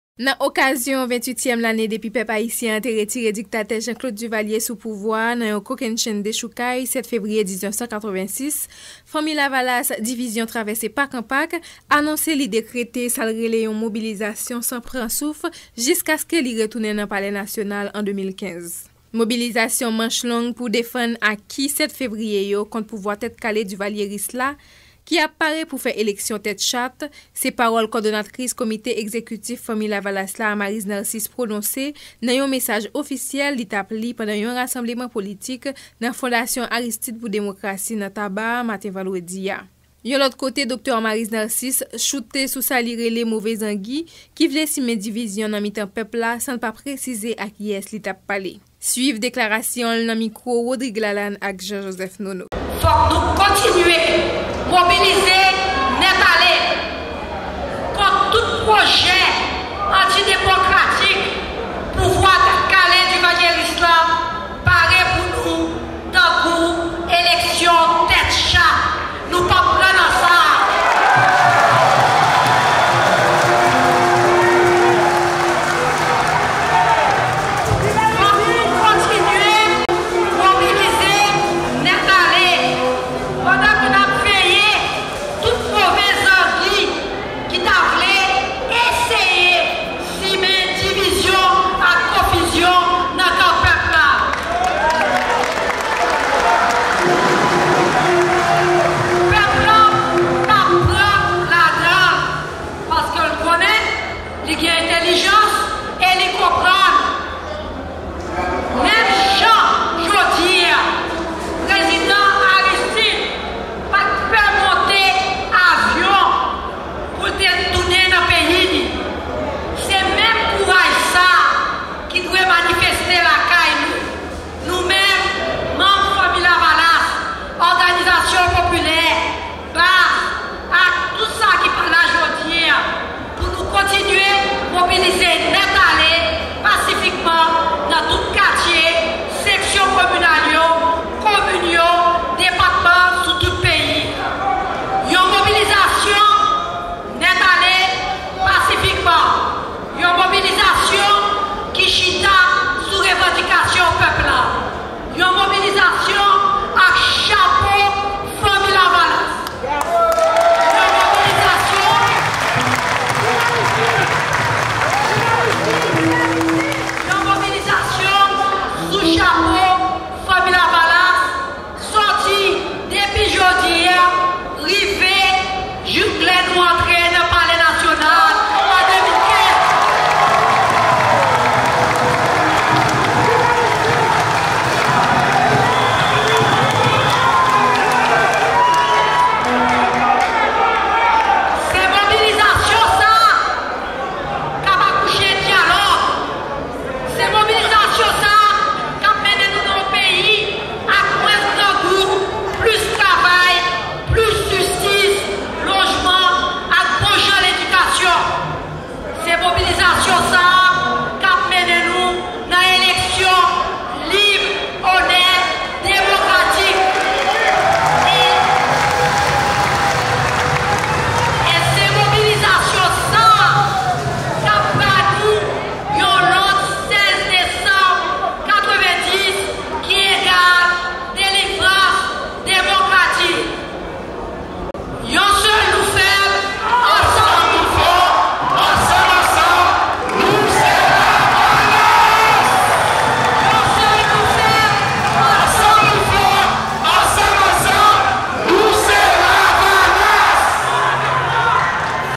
Commémoration du 7 février 1986: Fanmi Lavalas, tiraillé, décrète une "mobilisation générale"
À l'occasion des 28 ans de la chute du Dictateur Jean-Claude Duvalier, Fanmi Lavalas a tenu un rassemblement politique à la Fondation Aristide pour la Démocratie à Tabarre ce vendredi 8 février 2014. La Coordonnatrice de Fanmi Lavalas, Dr Maryse Narcisse a décrété une "mobilisation générale" jusqu'à un éventuel retour du parti au Palais National en 2015.